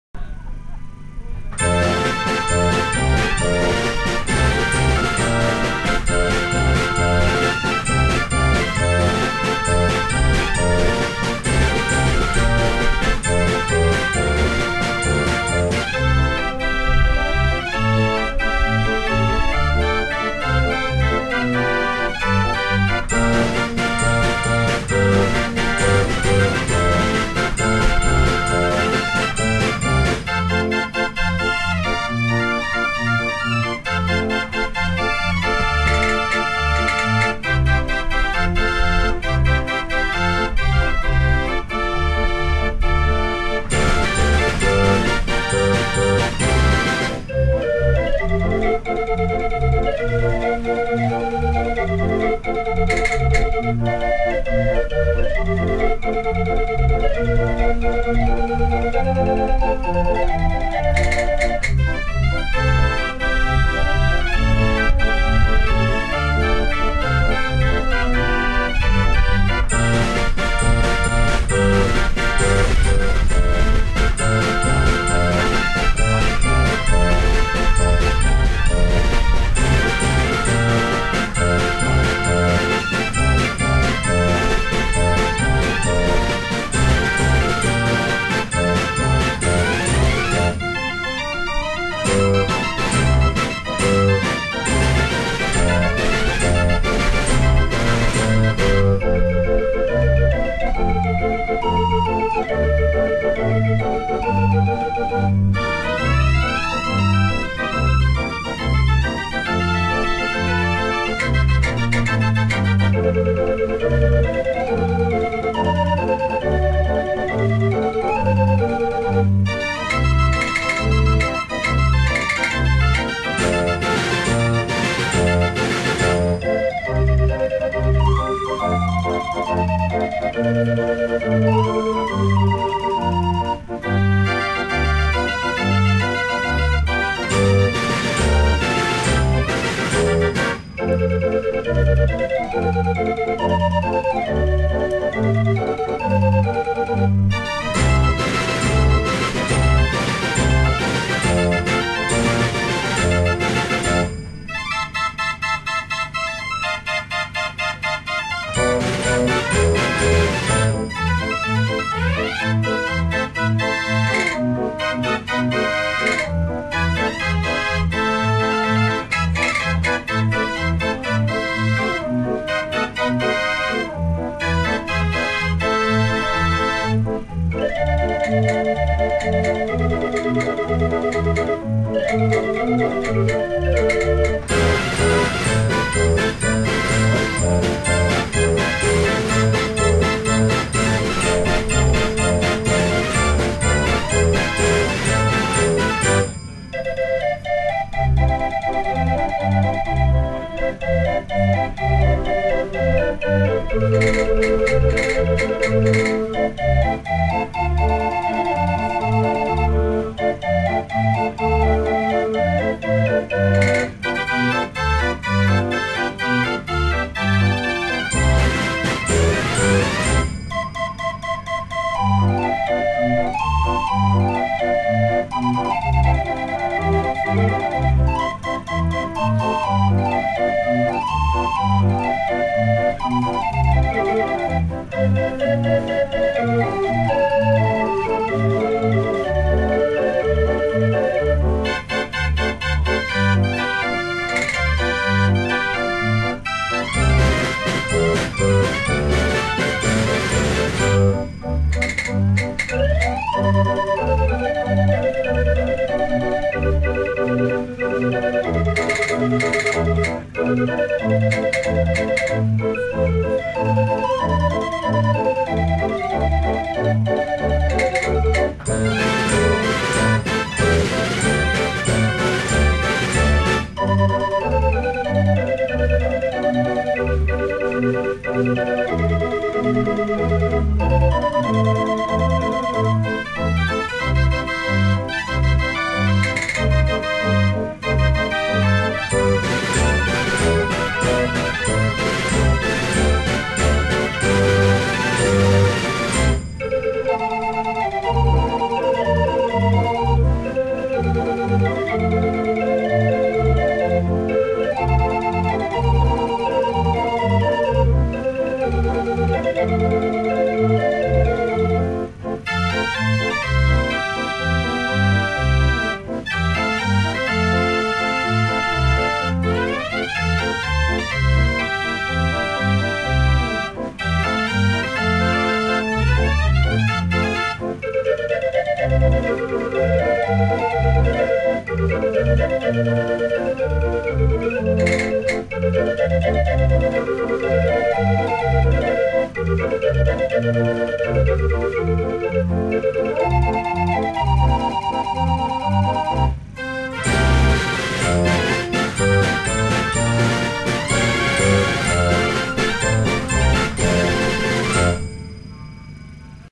ABBA Medley, played by Het Kleine Juweel
Audio and photo's from the "Organ Enthusiasts Day 2014" at Cotton Mechanical Music Museum, UK.
Fairground, Dance and Street Organs, Pianolas, Music Boxes, Orchestrions & more.